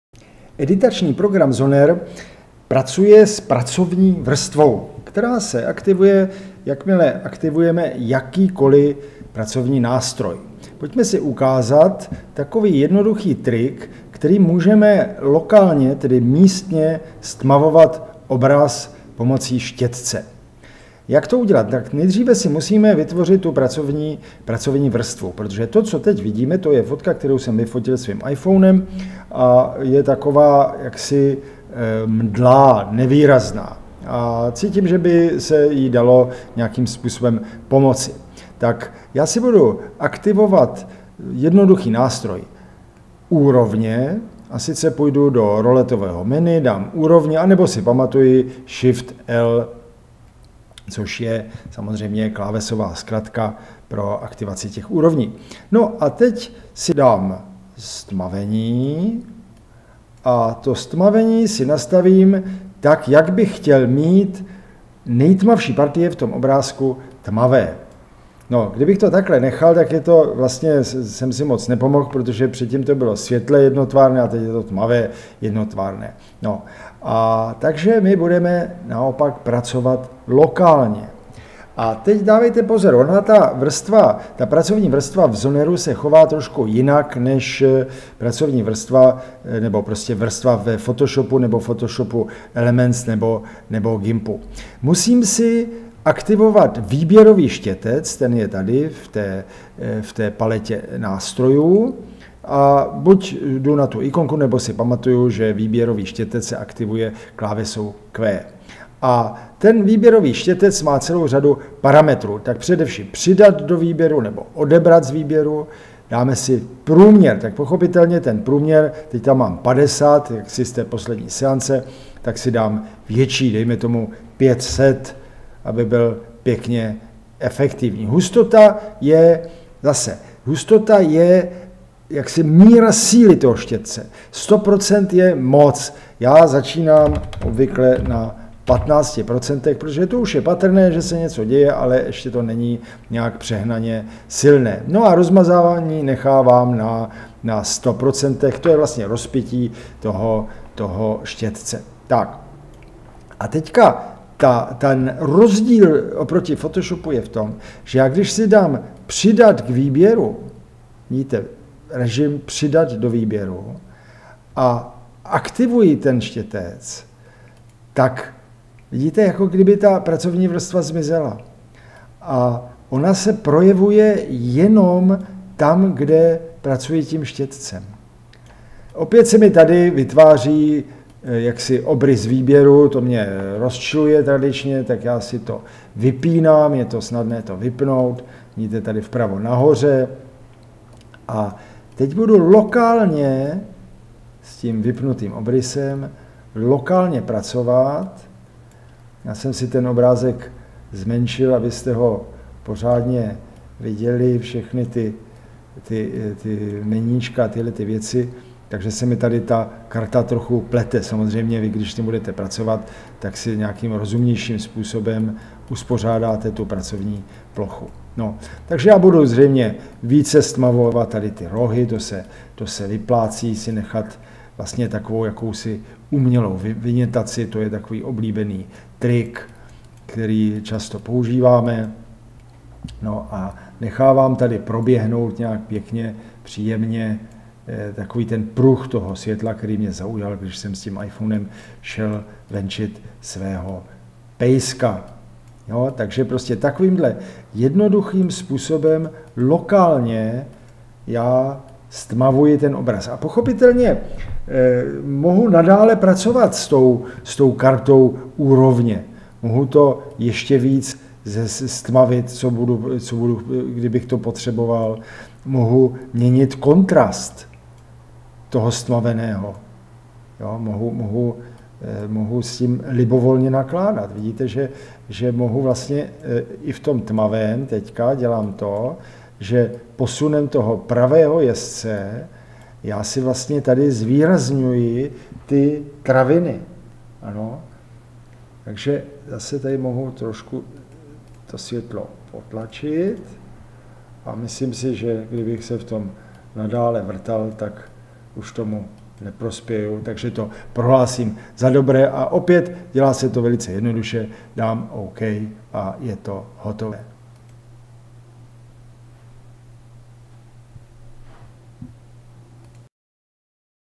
Lokální ztmavování ve vrstvách Zoneru
V režimu Přidat k výběru se v Zoneru mění štěte výběru v účinný efektový štětec.